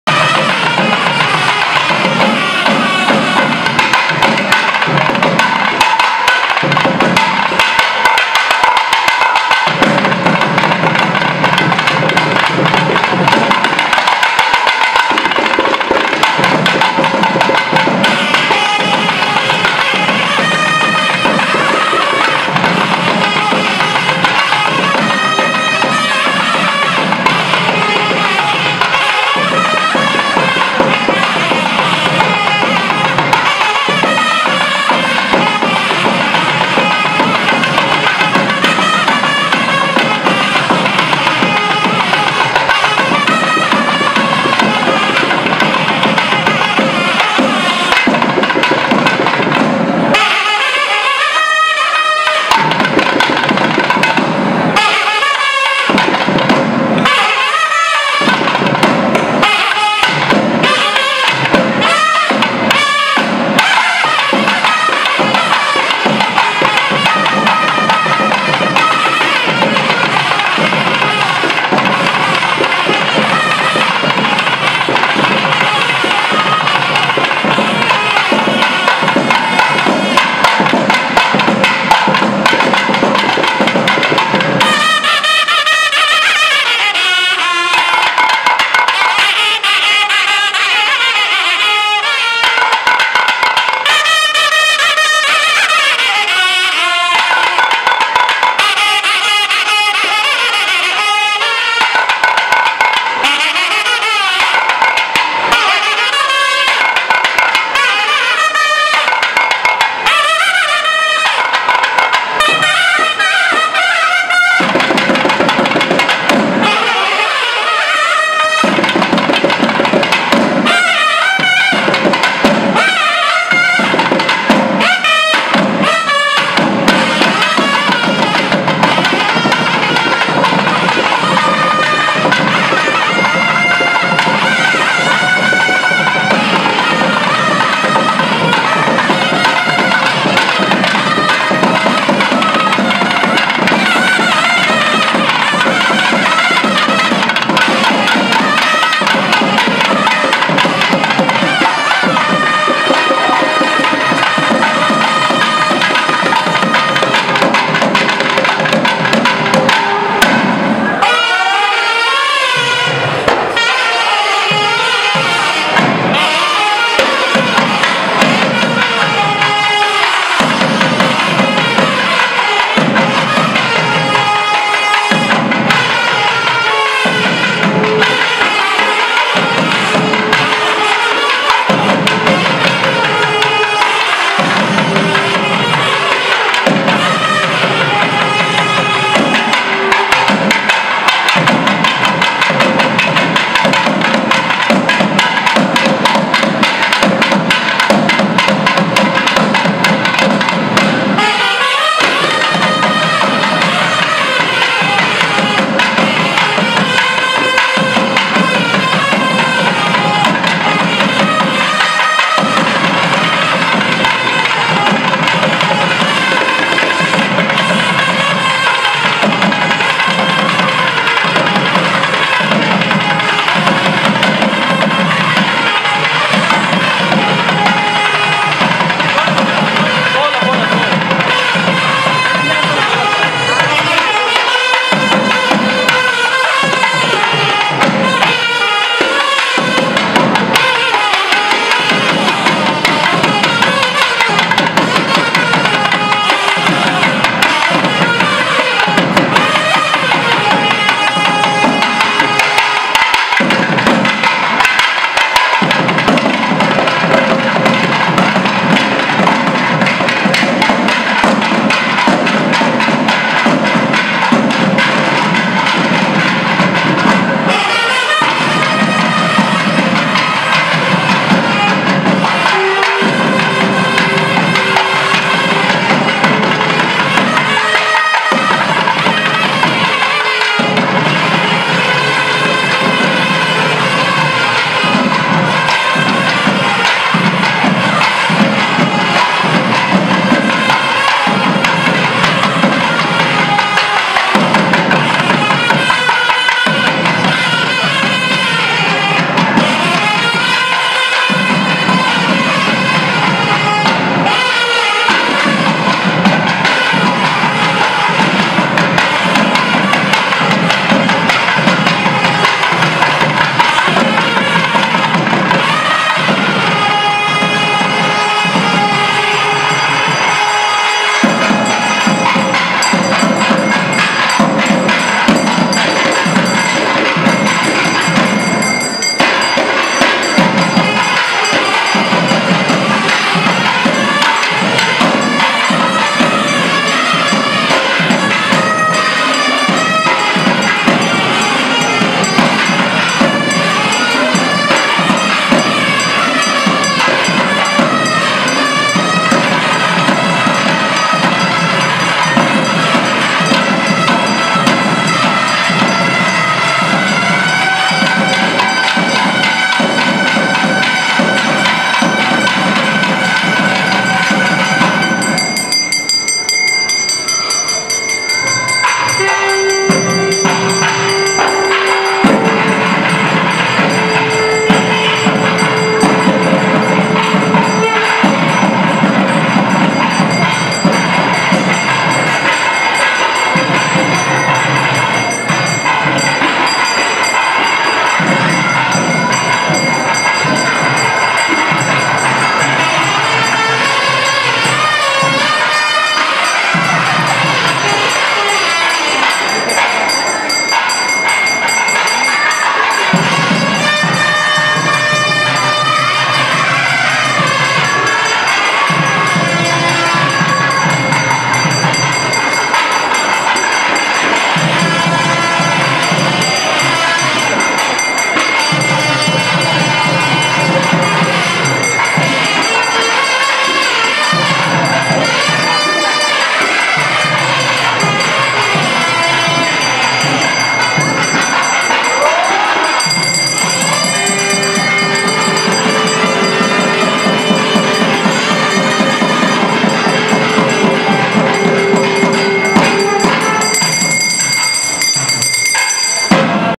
அருள்மிகு செண்பக விநாயகரின் எட்டாம் நாள் மாலை சப்பற திருவருட் காட்சி
ஏவிளம்பி வருட மஹோற்சவம் - எட்டாம் நாள் மாலை சப்பற திருவருட் காட்சி
HeiVilambi Year Mahotsavam - Eight Day Evening Sappara Festival.
07/06/2017